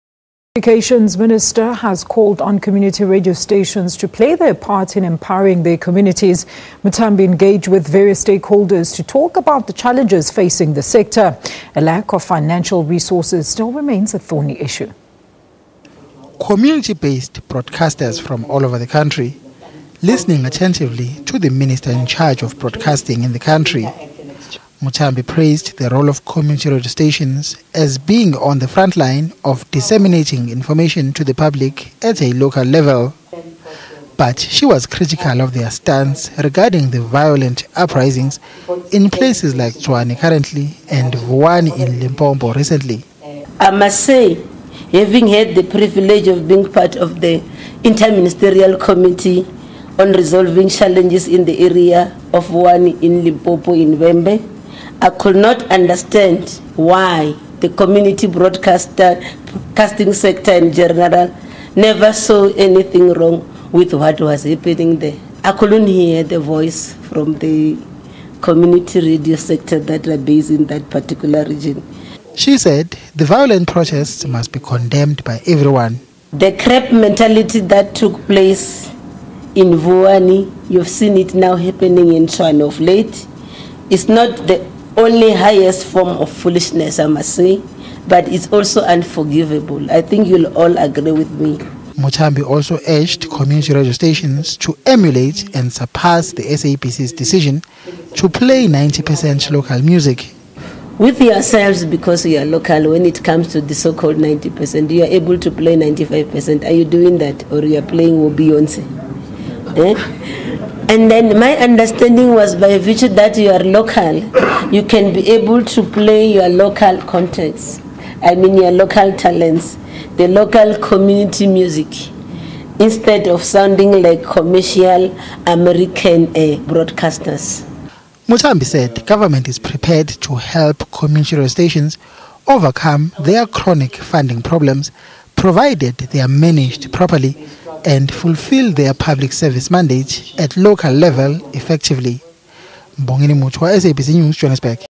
Community radio stations must empower communities: Faith Muthambi
The communications minister has called on community radio stations to play their part in empowering their communities. Faith Muthambi engaged with various stakeholders to talk about the challenges facing the sector. A lack of financial resources - remains a thorny issue.